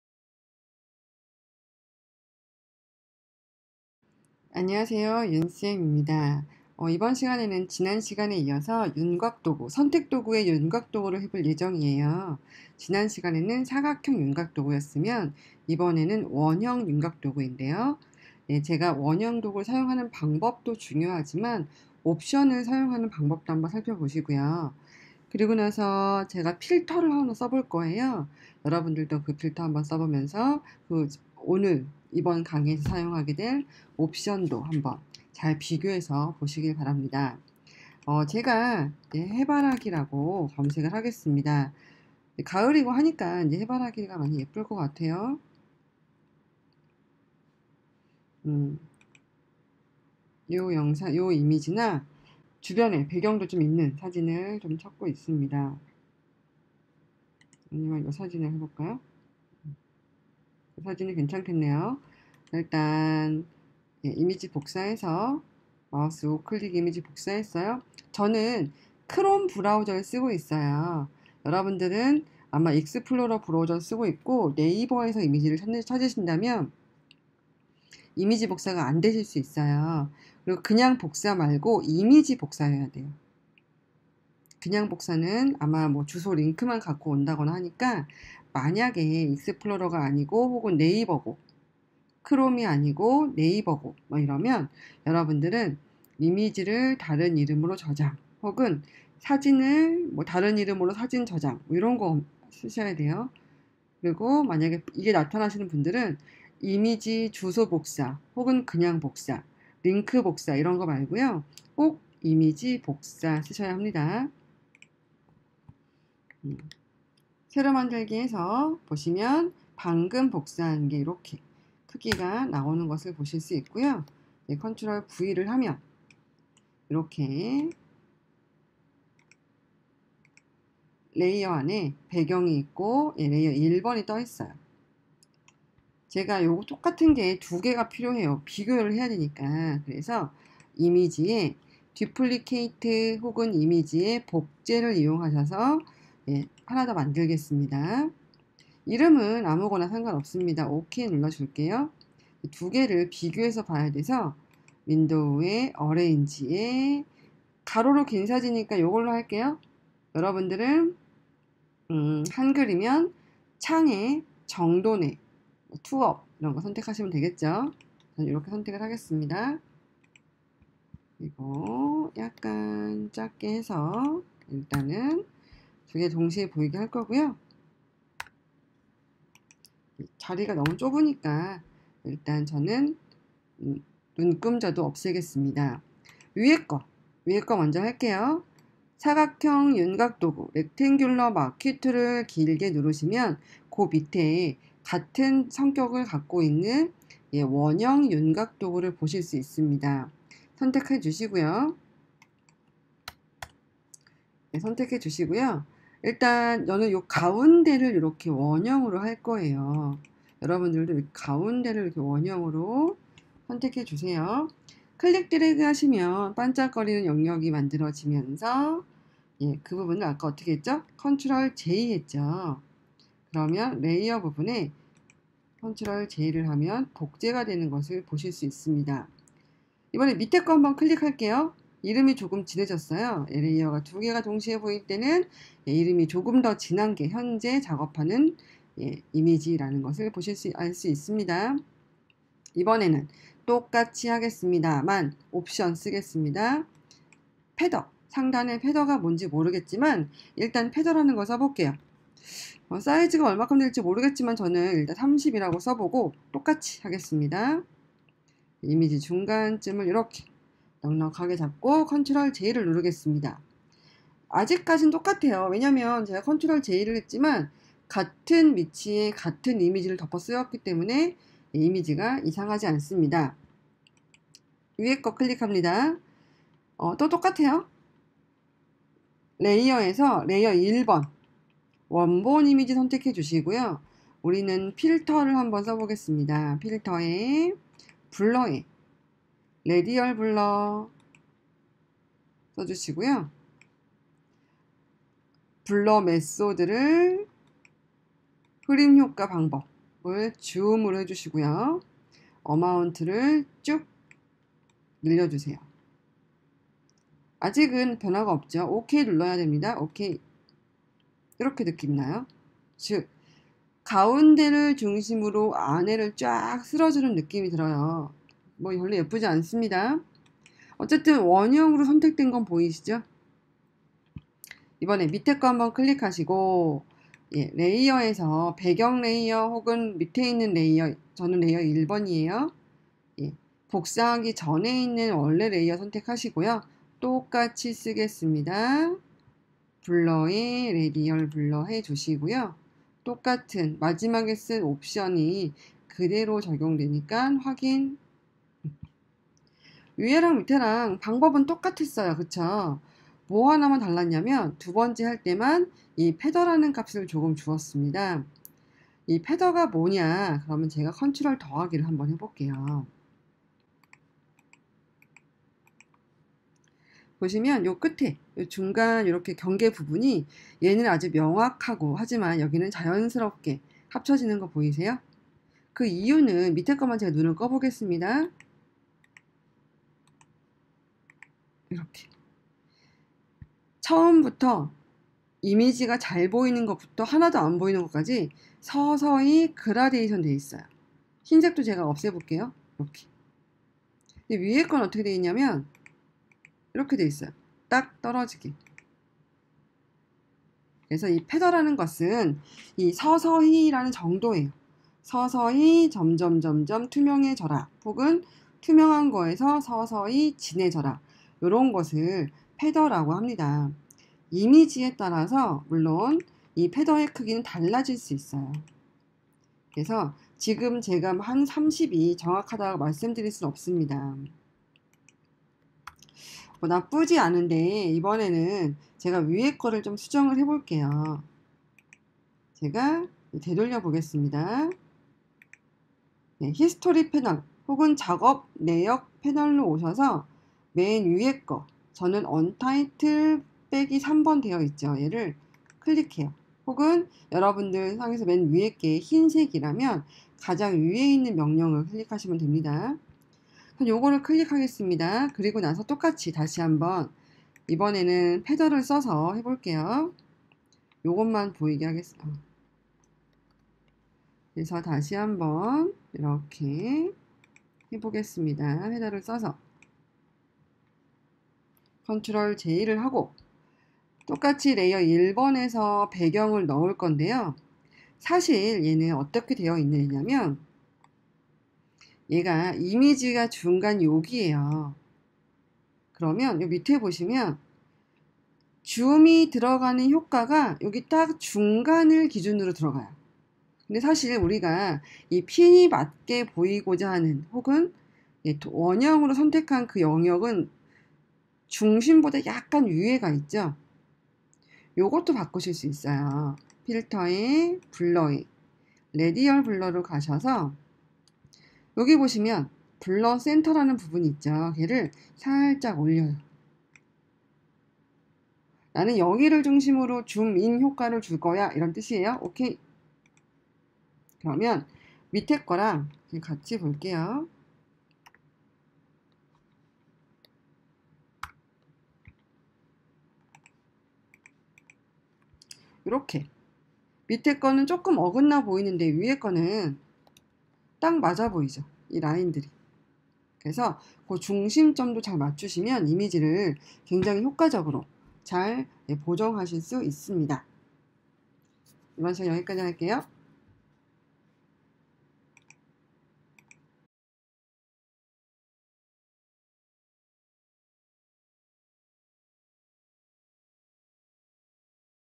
기초를 배우는 프로그램 입문서 - 포토샵/ 원형 선택도구, 윤곽도구 marquee tool
포토샵에서 선택도구는 매우 중요합니다.
잘 선택하고 잘 합성하고.. ^^

원형선택도구 사용방법과 패더의 이해 그리고 예제까지 이어지는 강의를 보면서 그 맥락을 잡아보아요..